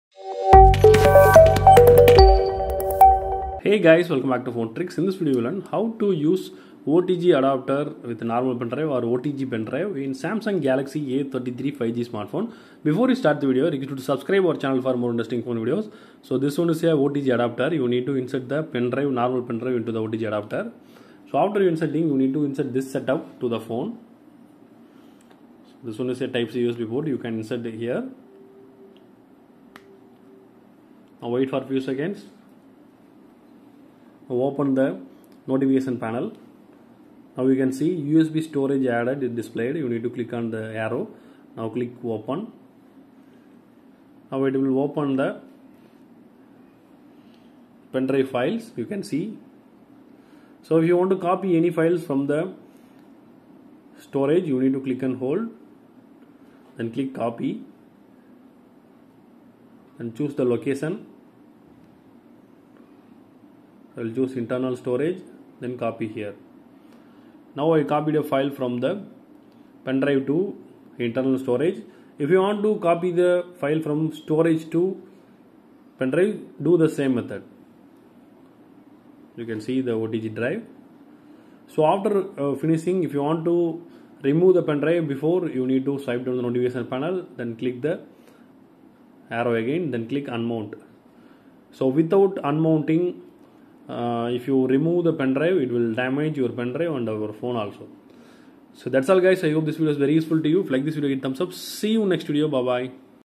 How to use OTG and OTG Pen drive in Samsung Galaxy A33 5G
Hey Friends  ! Checkout this new video of How to use OTG and OTG Pen drive in Samsung Galaxy A33 5G